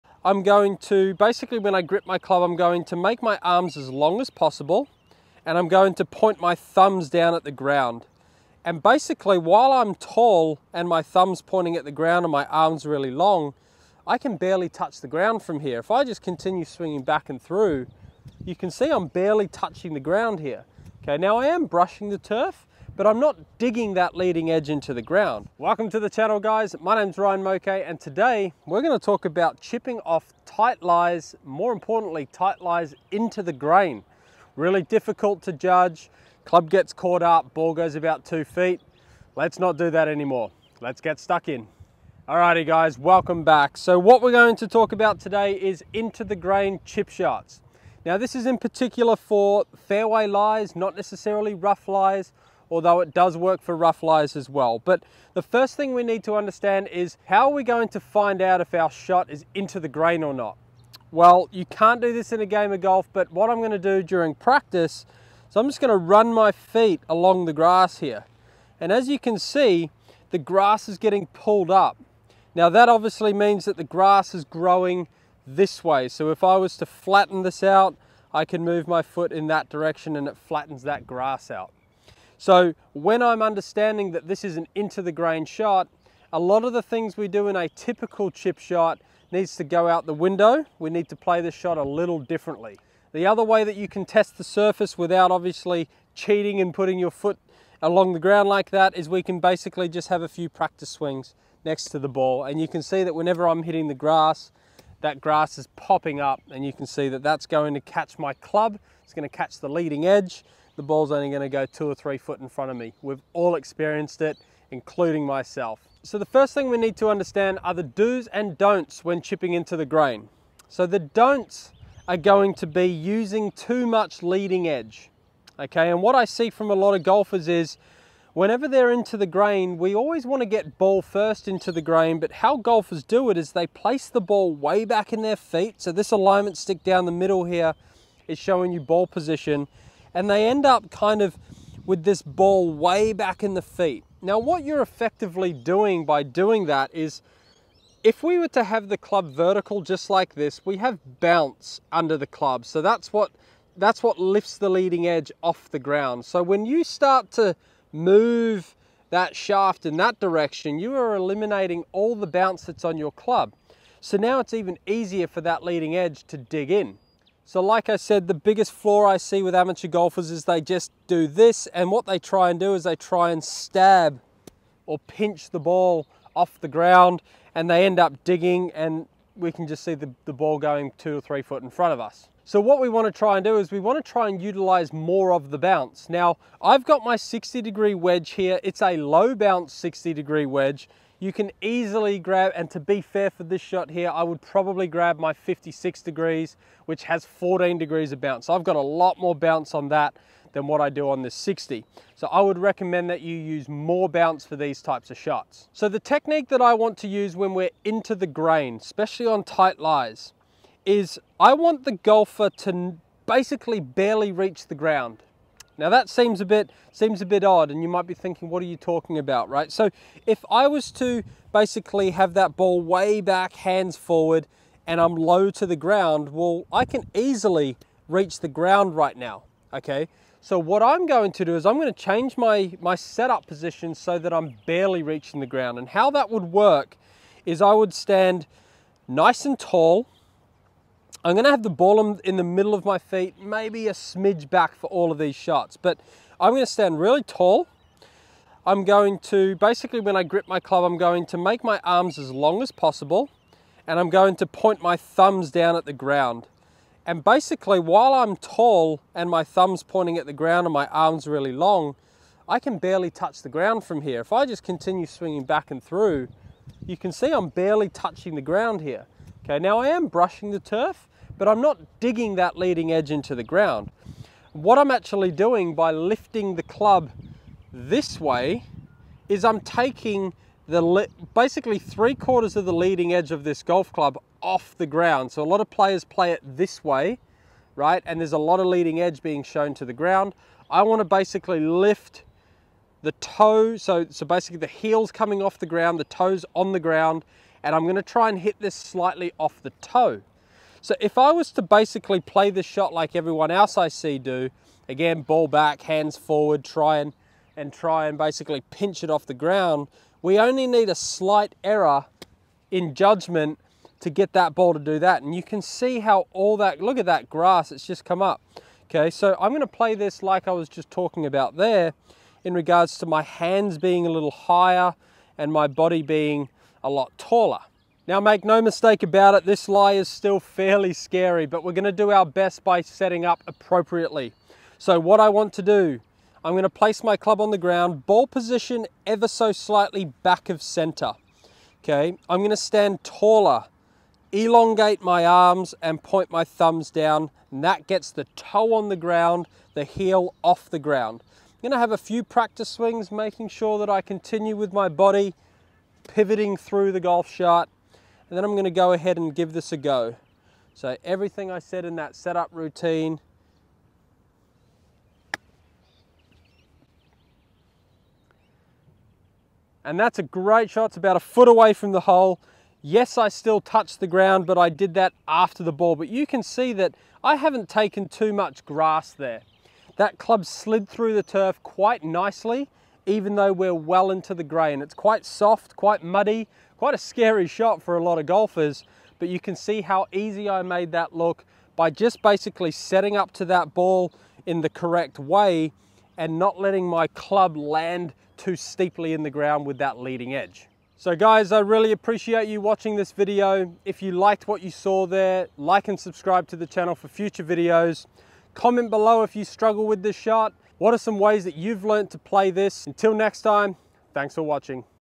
DO THIS When Chipping Into The Grain | Stop Blading and Duffing!
Chipping into the grain is a NIGHTMARE for 99.9% of golfers on the planet. In this video, Ryan shows you some common mistakes the recreational golfer makes when faced with this shot, and what you can do instead to give yourself the best chance at hitting a good shot from these tricky lies around the green.